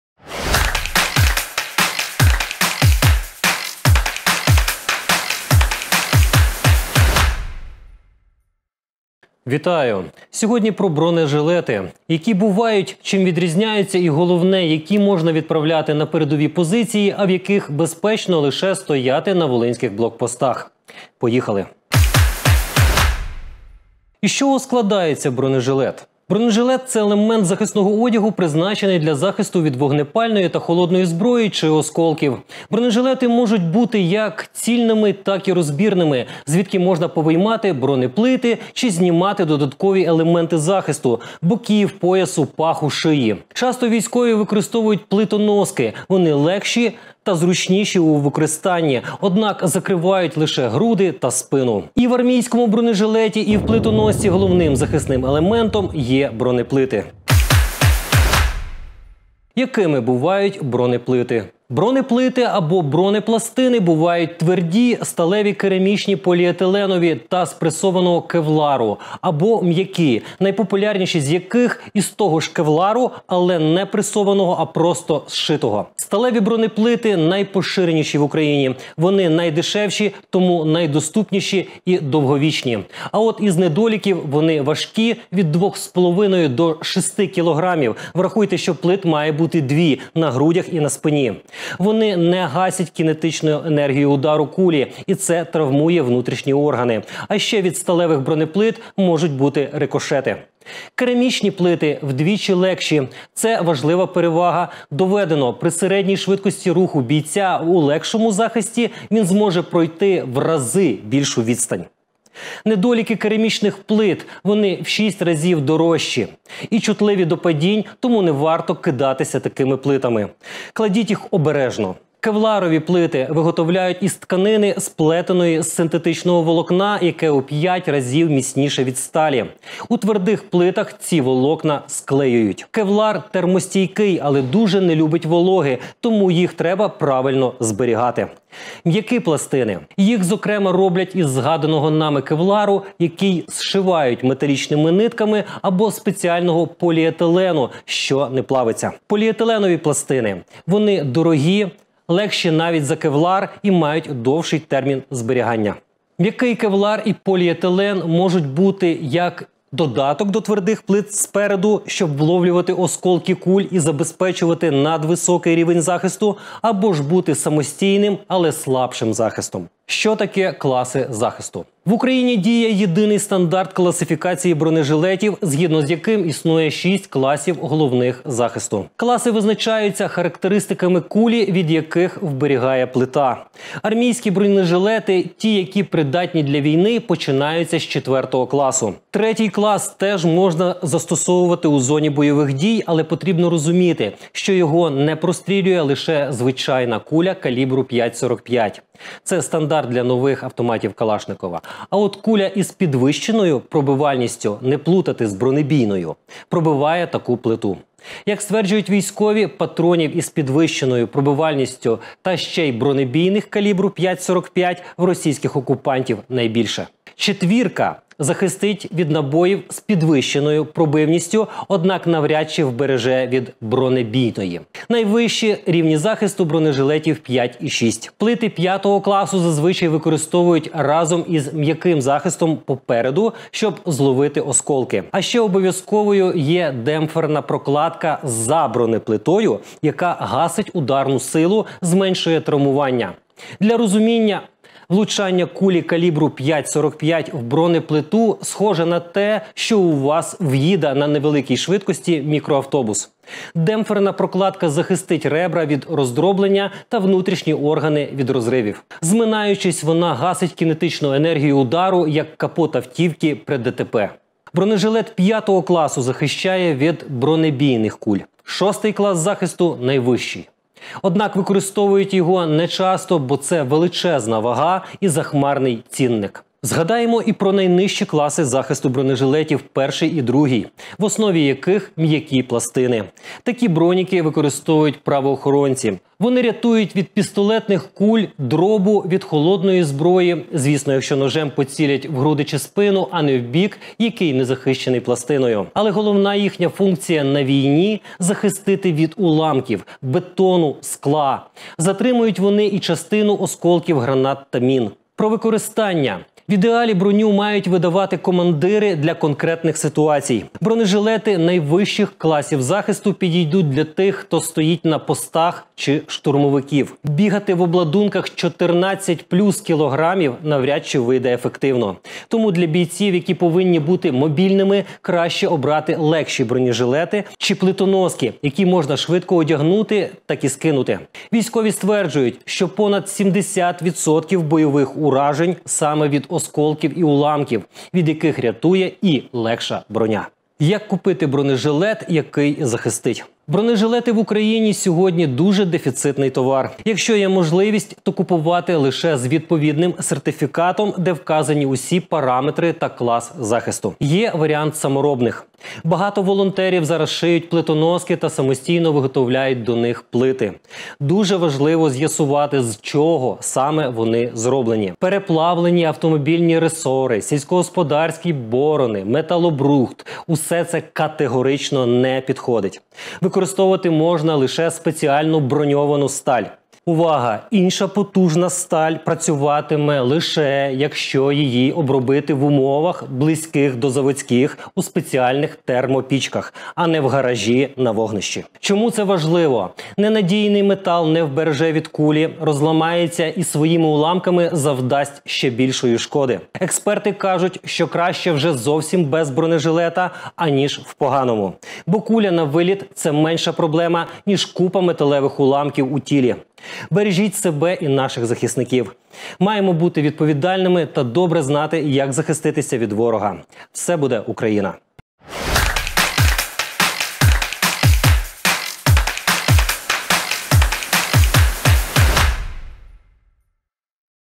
Як вибирати бронежилет | На пальцях
Як вибрати бронежилет, які вони бувають і в чому їхня різниця. Пояснюємо НА_ПАЛЬЦЯХ про найголовніше в цій темі